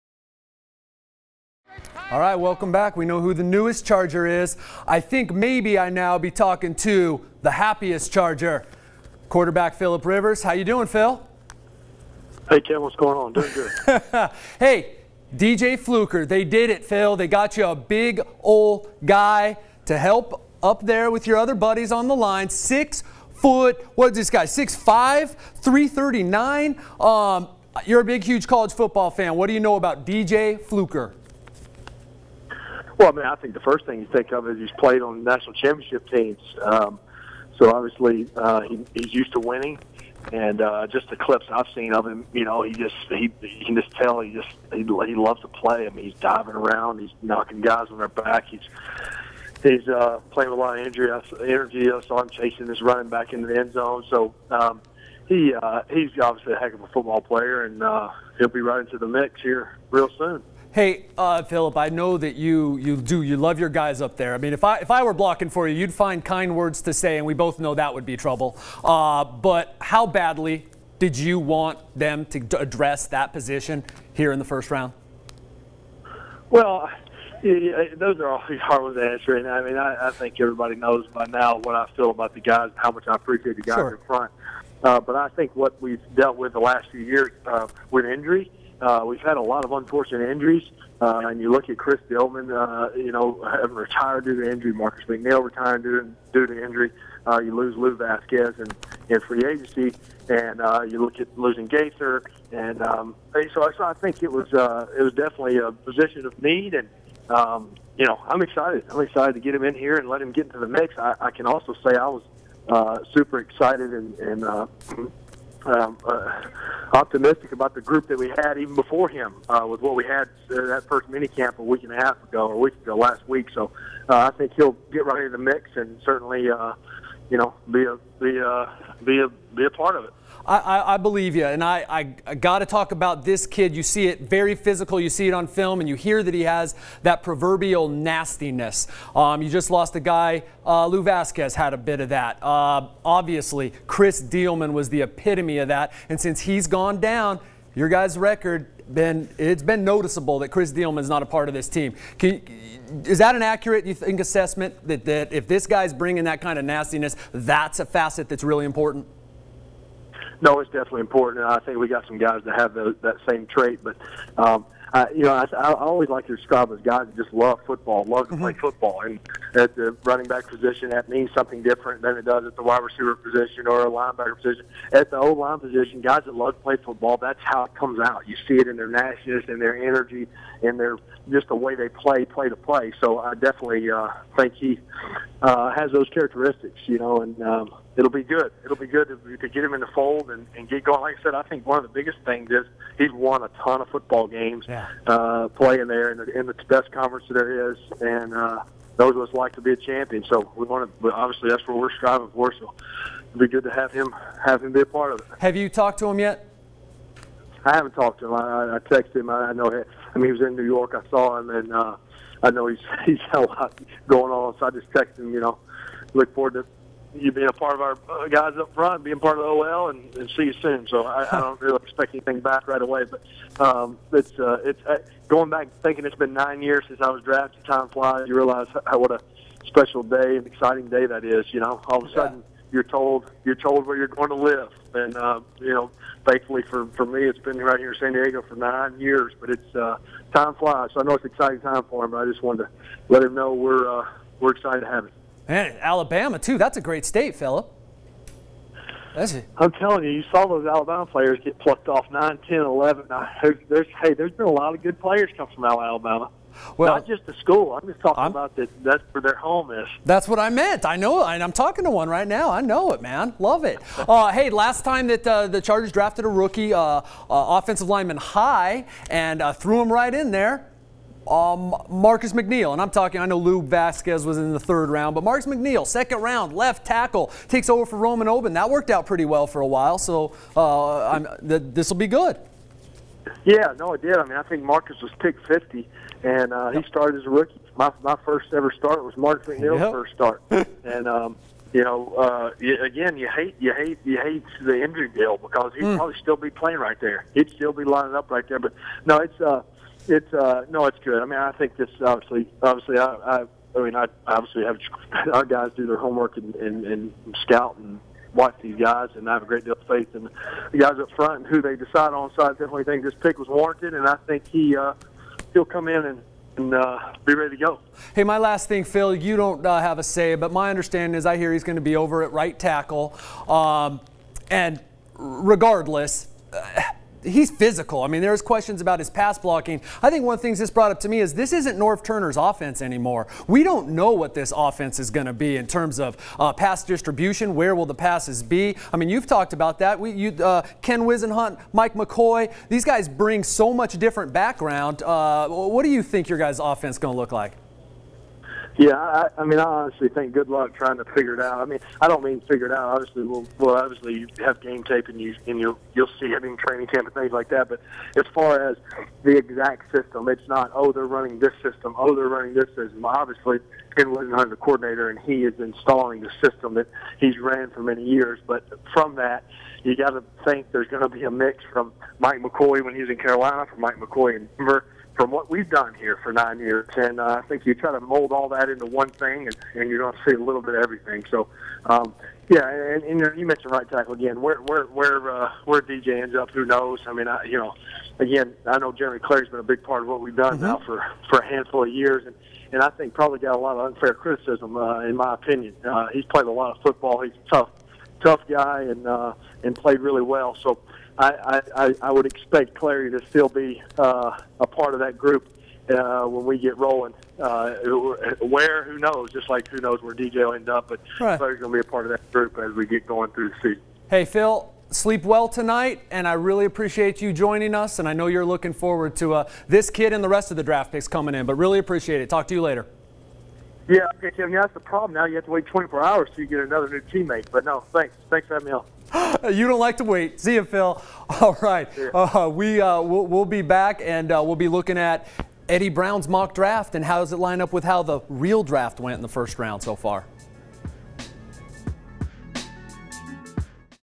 Philip Rivers on D.J. Fluker
Philip Rivers phoner with Acee... talking Fluker